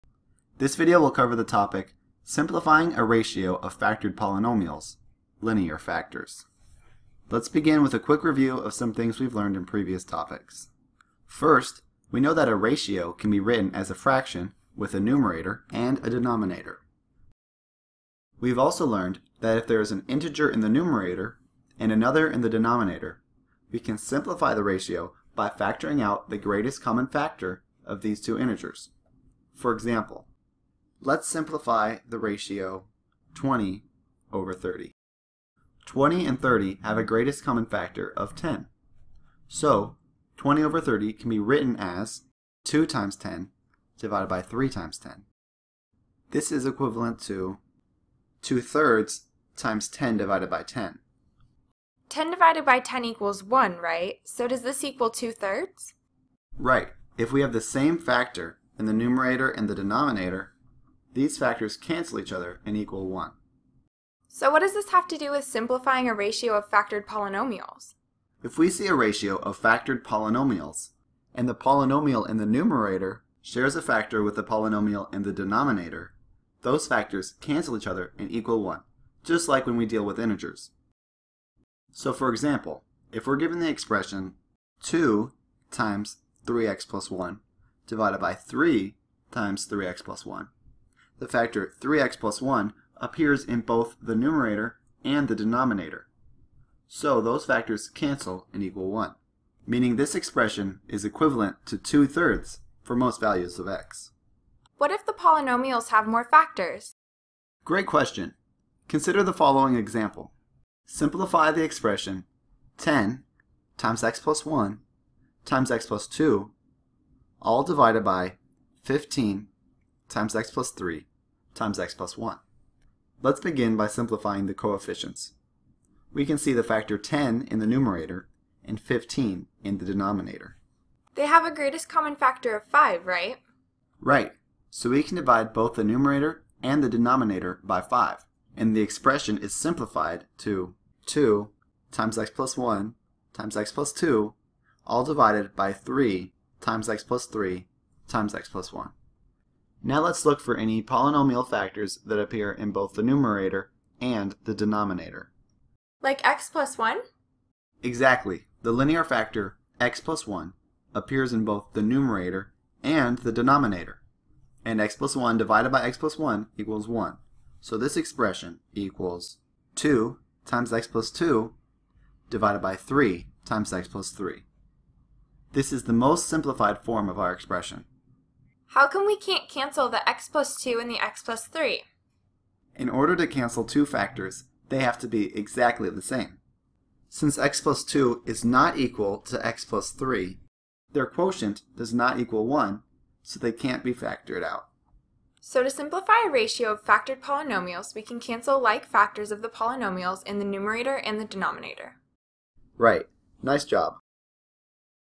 Topic: Simplifying a Ratio of Factored Polynomials: Linear Factors
This is a topic level video of Simplifying a Ratio of Factored Polynomials: Linear Factors for ASU.

Join us!